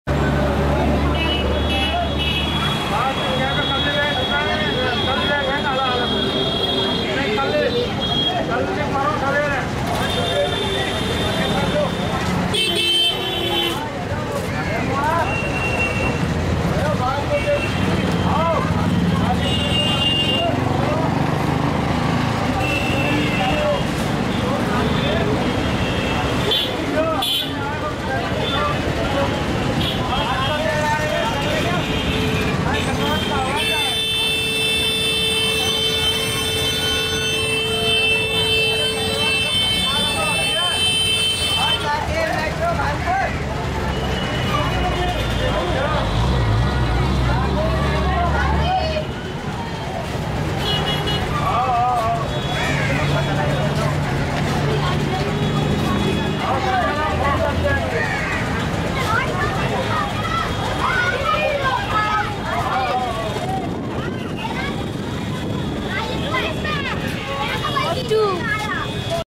Waiting time
old man and some other people are waiting for the bus at the bus stand.